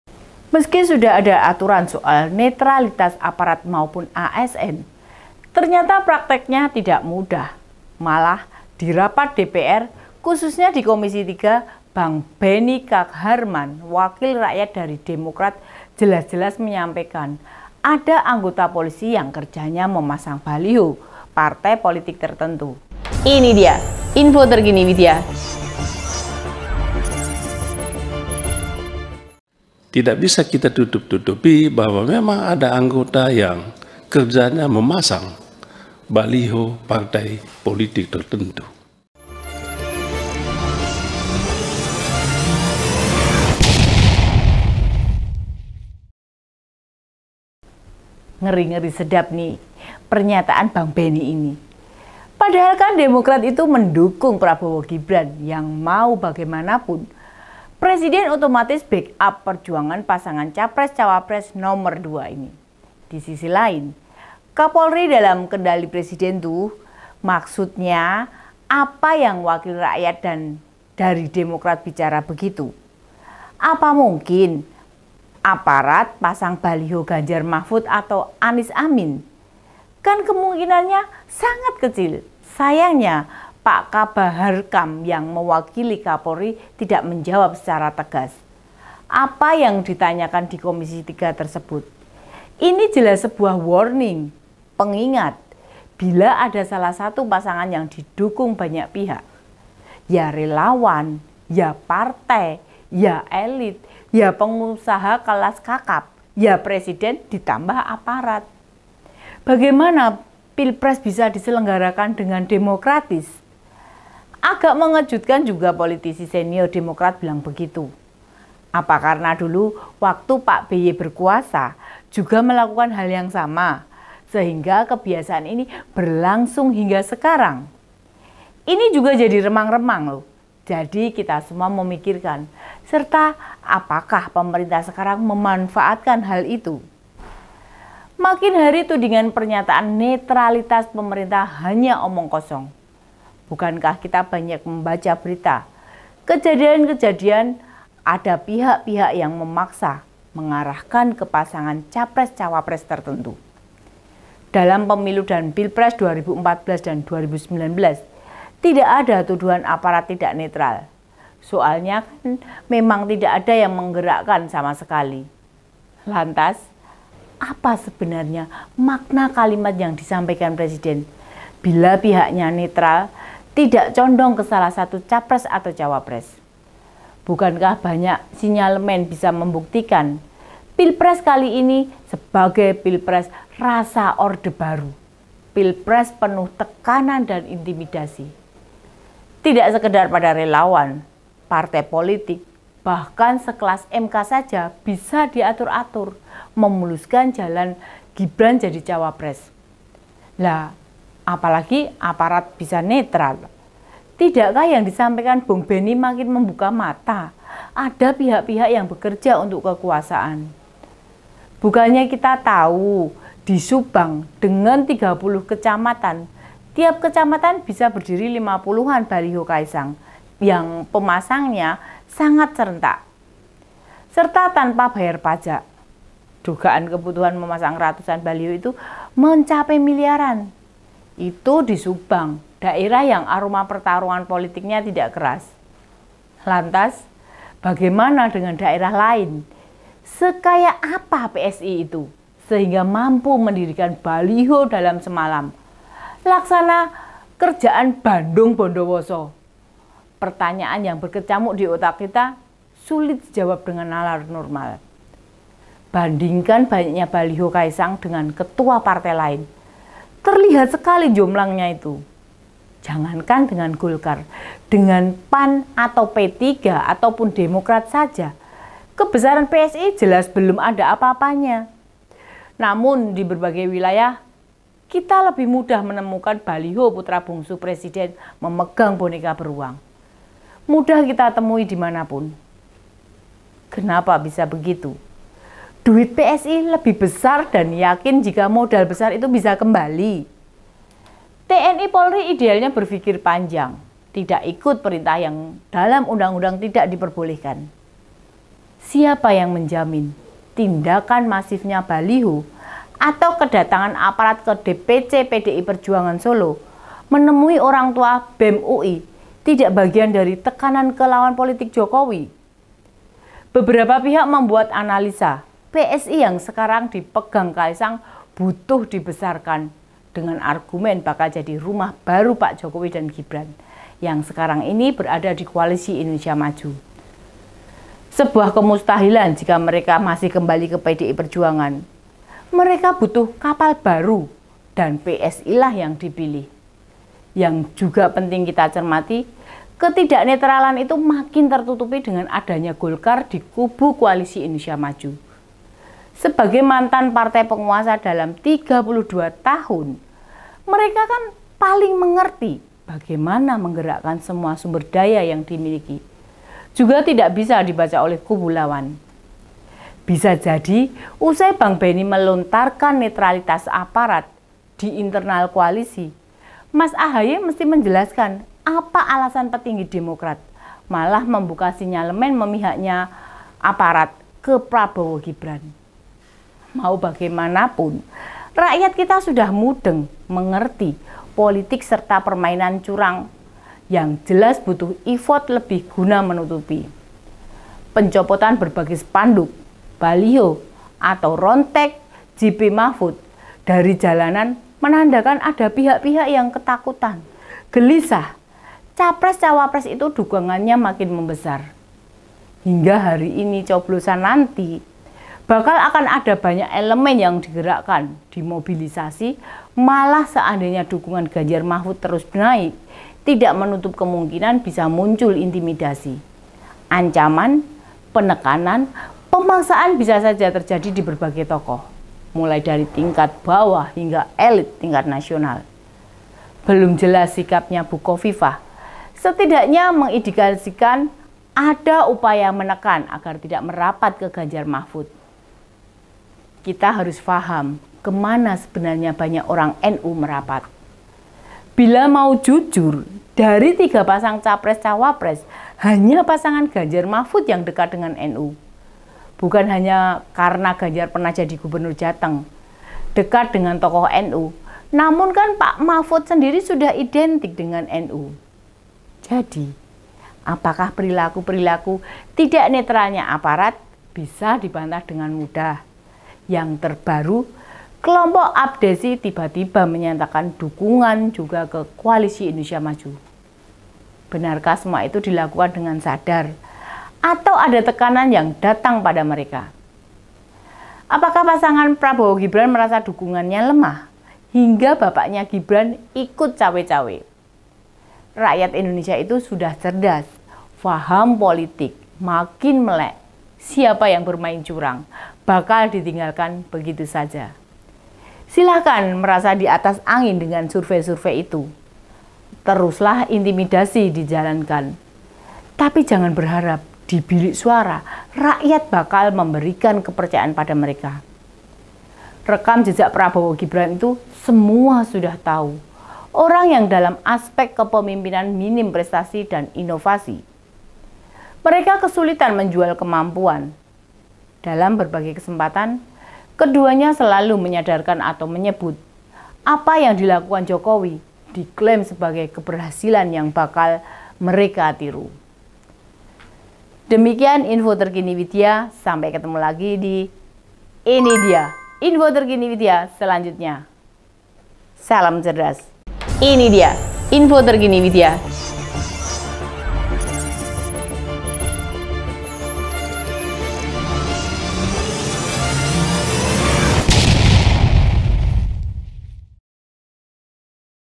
Ini Dya: CADAASS!! DIBONGKAR BENNY K HARMAN ADA APARAT TAK NETRAL, KAWAN KOALISI AJA NGAKUIN?? #192
Sejumlah anggota Komisi III DPR mengingatkan Polri untuk menjaga netralitasnya sepanjang perhelatan Pemilu 2024. Anggota Komisi III DPR dari Demokrat, Benny K Harman bahkan menyebut netralitas aparat Polri masih menjadi sebuah utopia. Sebab, Benny menyebut saat ini ada anggota polisi yang kerjanya memasang baliho partai politik (Parpol) tertentu.